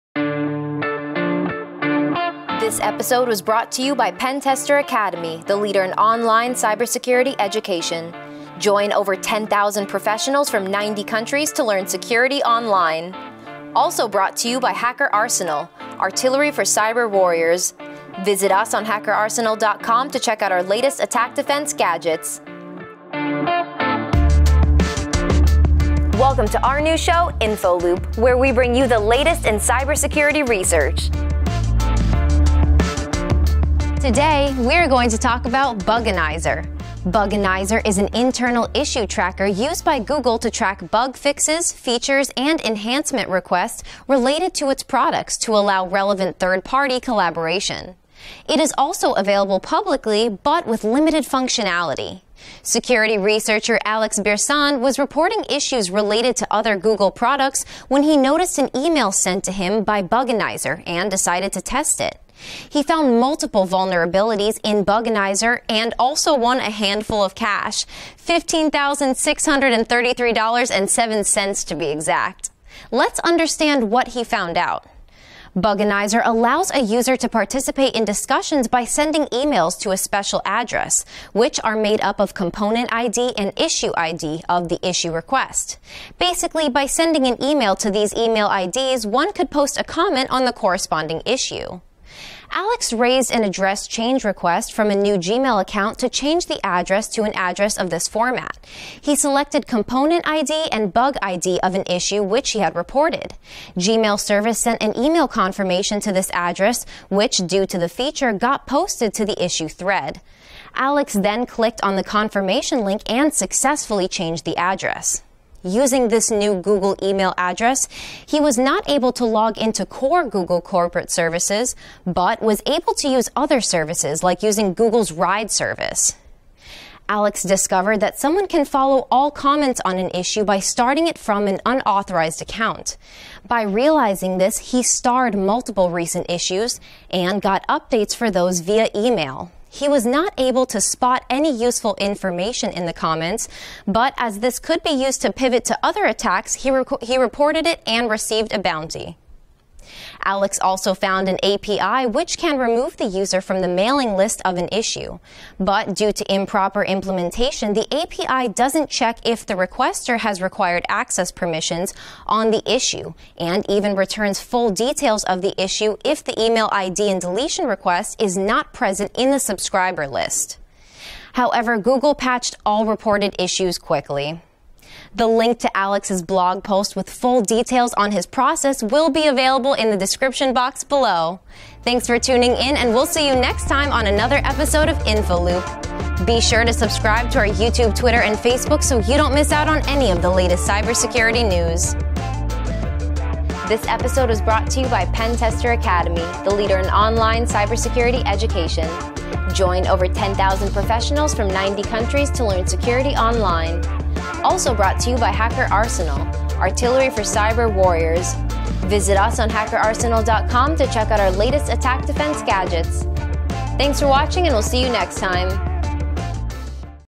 InfoLoop | Ep 3 | Targeting Buganizer
Today's episode of InfoLoop features Buganizer. We discuss the latest in cybersecurity research as well as the next great advancements in the security industry.